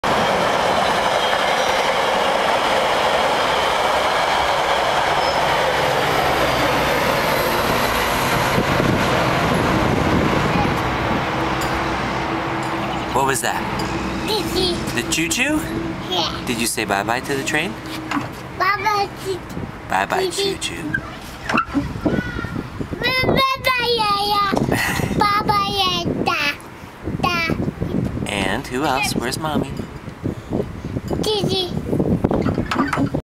Life Imitates Playground
While Henry Jay plays the role of the engineer on the train in the park, a real train goes by.